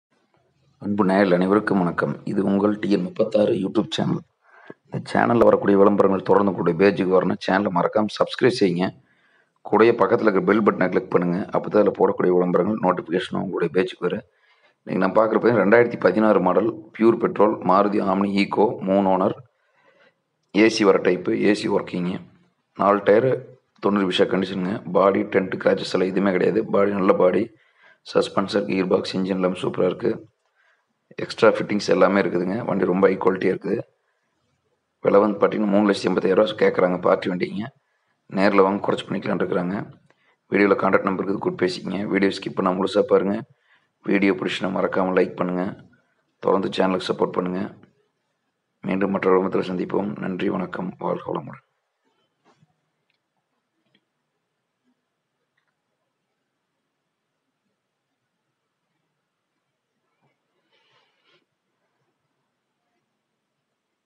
2016 ஈகோ விற்பனை
2016 மாருதி ஈகோ விற்பனை

Pure petrol
Ac working
New seat covers
No replaced
Hi quality
Own3
Full fittings
Rate:3,85,,000/asking
லோன் வசதி உண்டு
அனைத்து வியாபாரம் கமிசன் அடிப்படை

தொடர்புக்கு
பொதிகை சுந்தர்
96/98/55/99/99.
கோபிசெட்டிபாளையம்